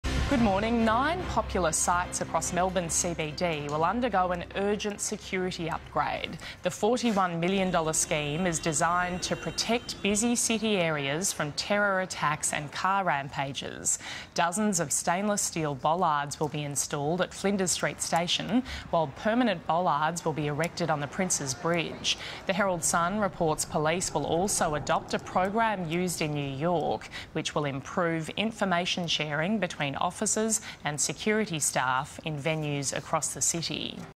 9 popular sites in Melbourne's CBD will undergo an urgent $41M terrorist security upgrade
Nine popular sites across Melbourne's CBD will undergo an urgent security upgrade. The $41M scheme is designed to protect busy city areas from terror attacks and car rampages.